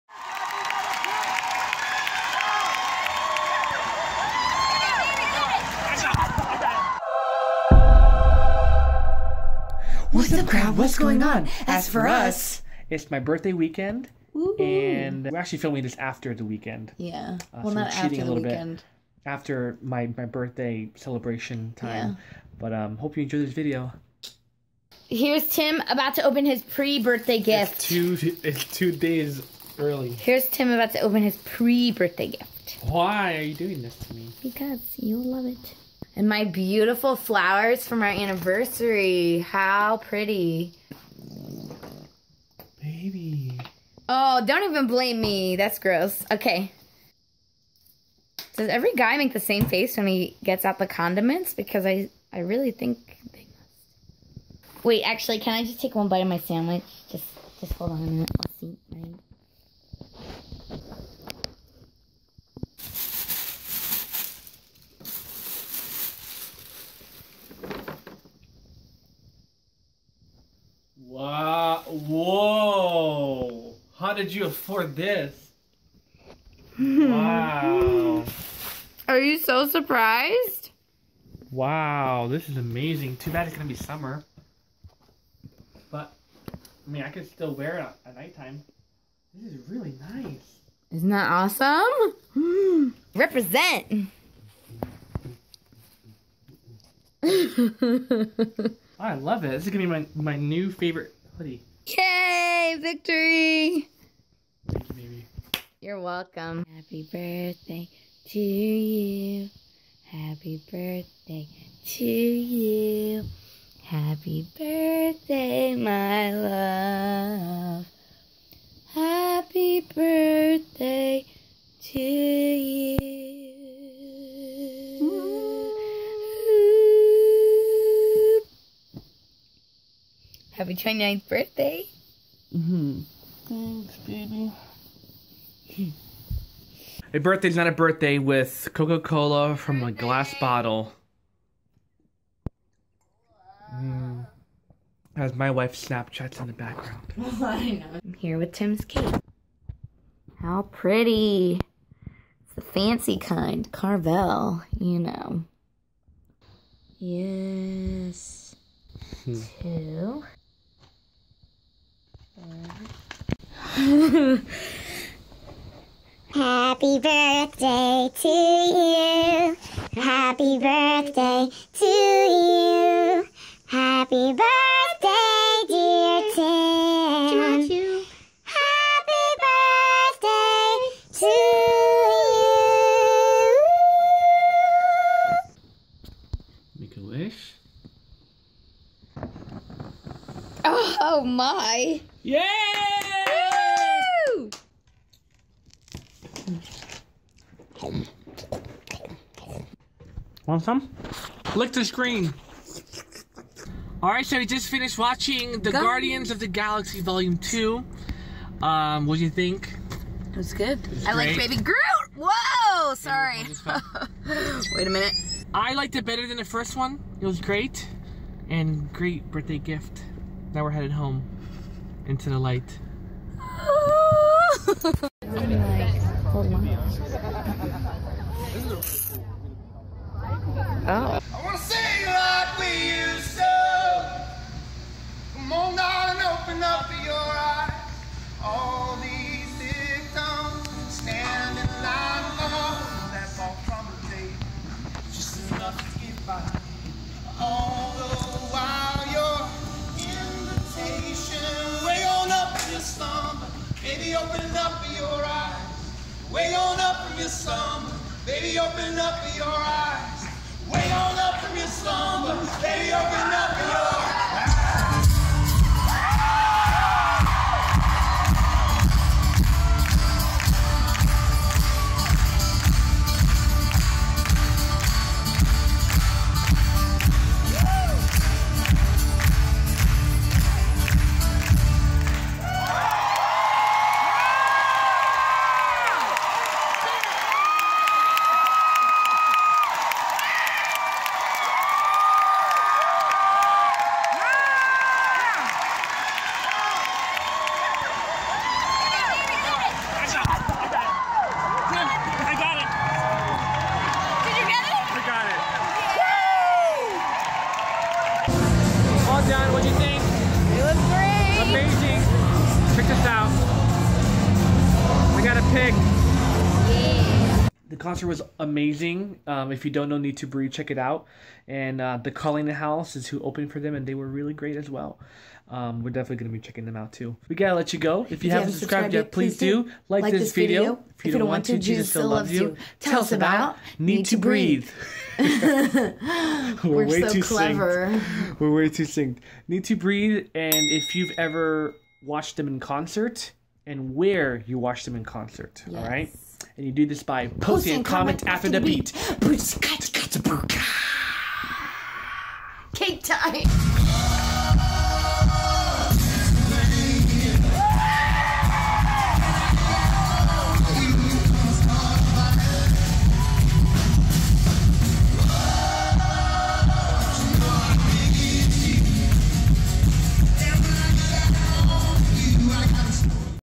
NEEDTOBREATHE Hard Love Concert | FAN GRABS GUITAR PICK!
NEEDTOBREATHE started their Hard Love tour on May 5th, 2017 in Bethlehem, PA, and guess what? We were there to see it for Tim's 29th birthday celebration! Also, a lucky fan was able to get Seth's (bass player) pick. I wonder who that was! Check out the vlog to see!

NEXT VLOG ► SATURDAY!

o-o-o-o-o-o-o-o-o-o-o-o-o-o-o-o

Tim's Twitter ► @TimAzevedo
Melinda's Twitter ► @kelley_melinda